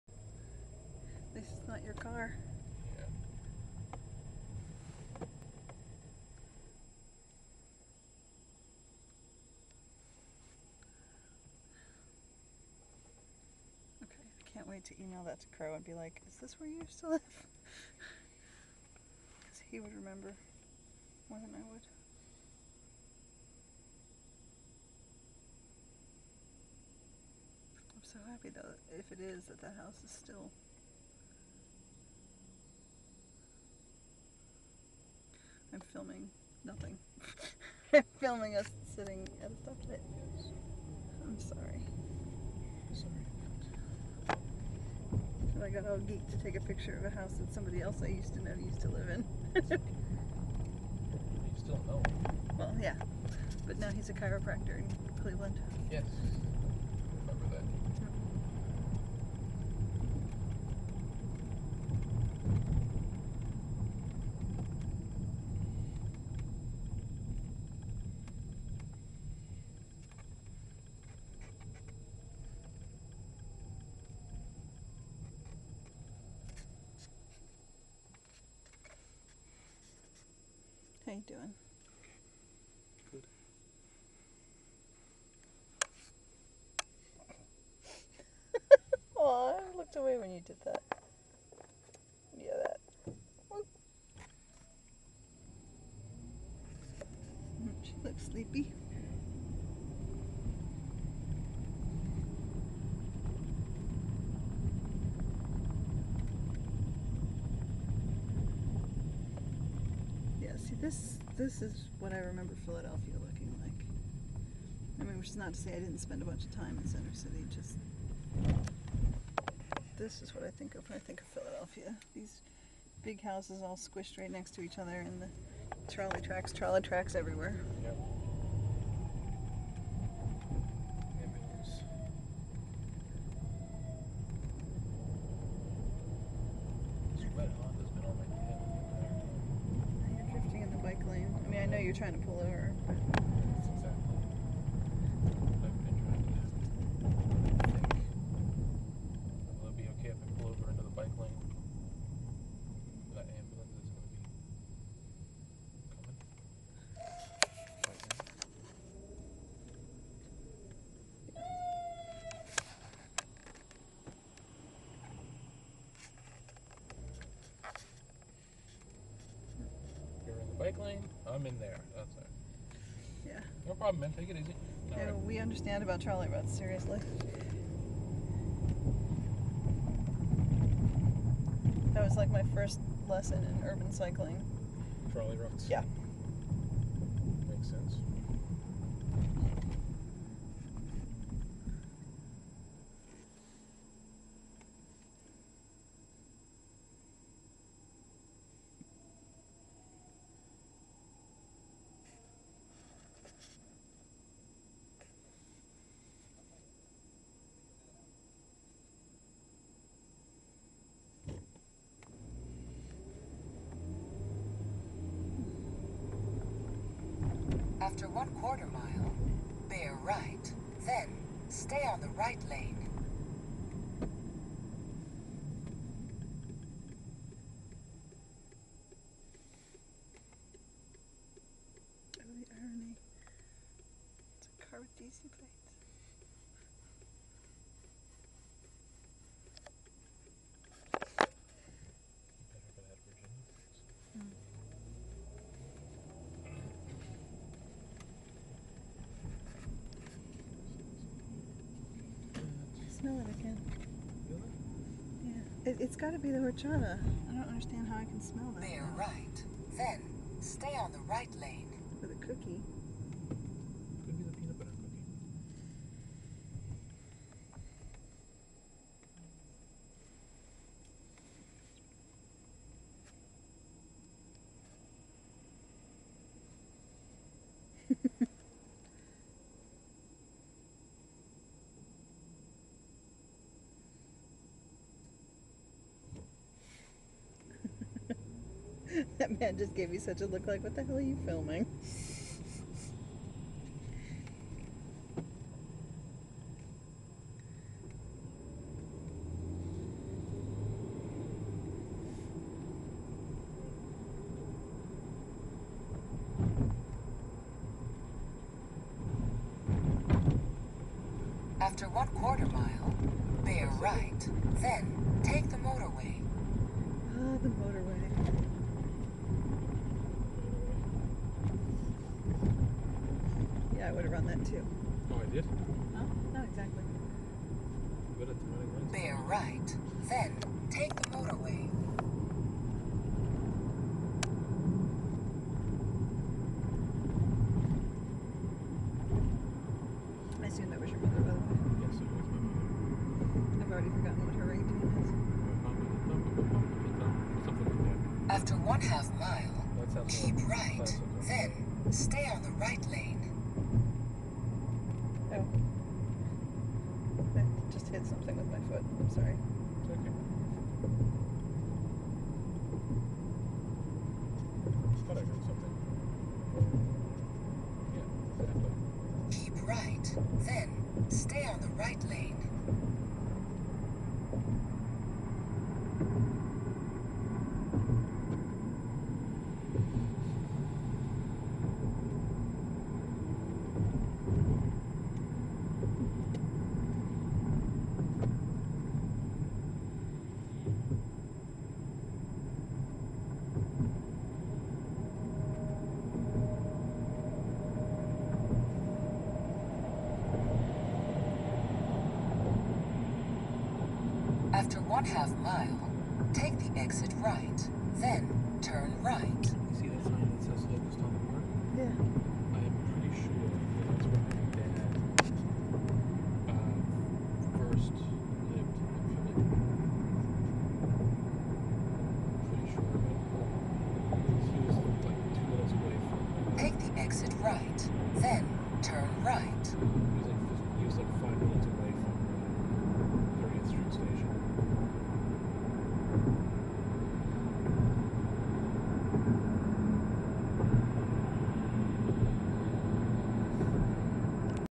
February 19, 2009 -- driving in Philadelphia with Aaron
Driving from West Phialdelphia to Center City with Aaron